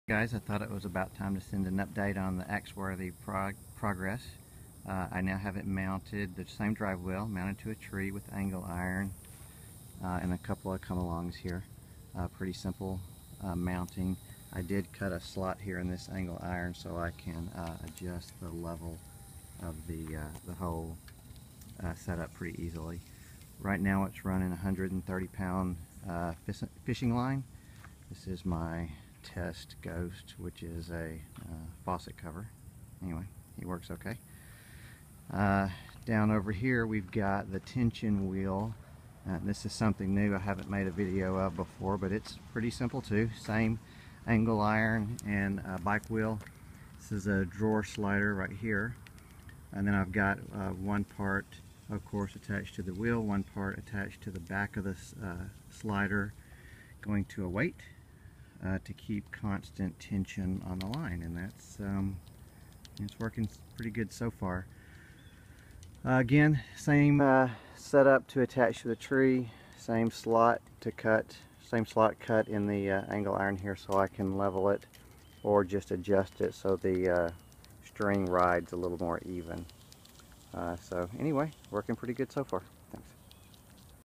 Axworthy Ghost 2014 - Trial Run
Trial outdoor run of Axworthy Ghost project.  Drive wheel is a wiper motor chain-linked to a kid's rear bike wheel.  Tension wheel is on drawer slider.